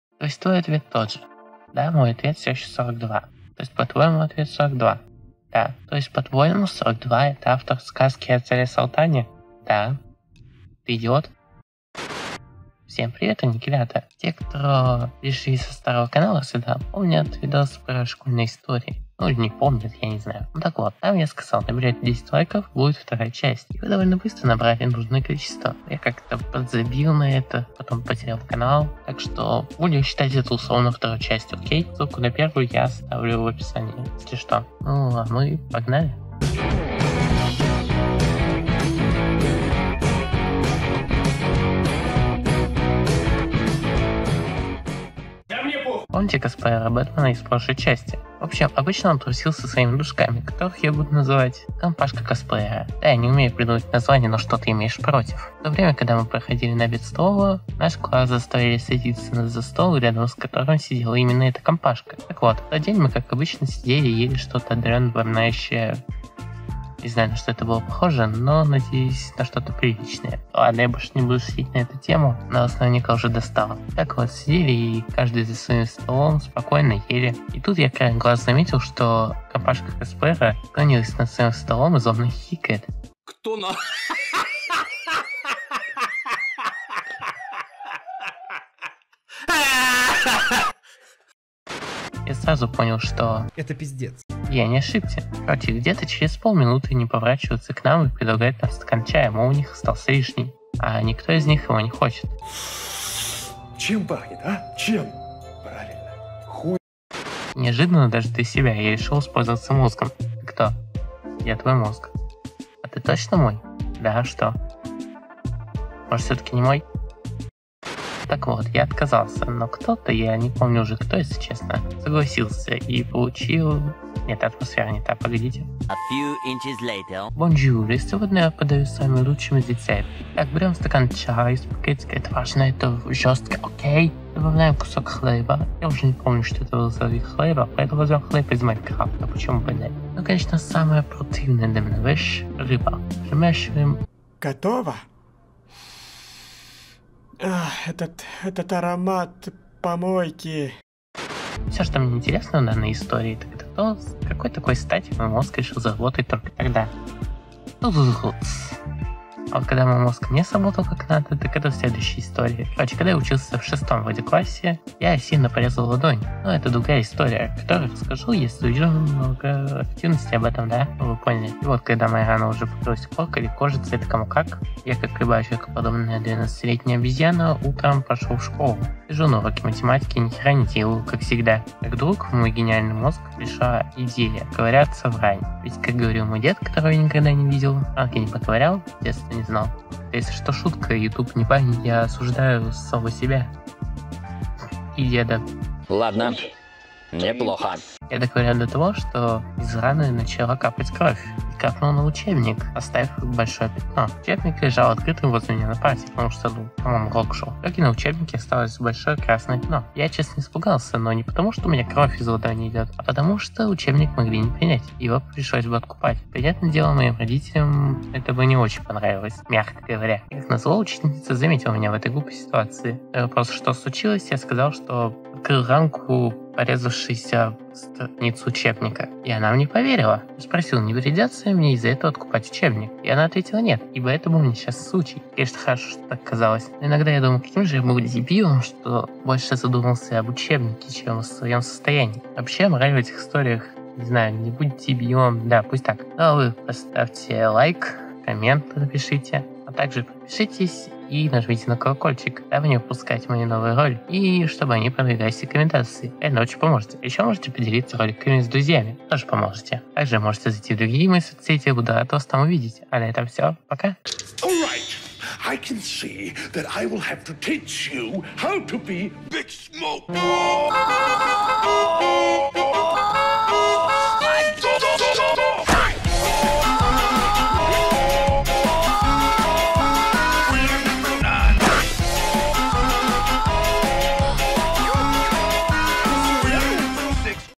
Вроде школьные истории
Сегодня я расскажу об историях из моей школы. Для кого-то это будет продолжение видоса со старого канала, а для кого-то-первые дурацкие истории из моей школы.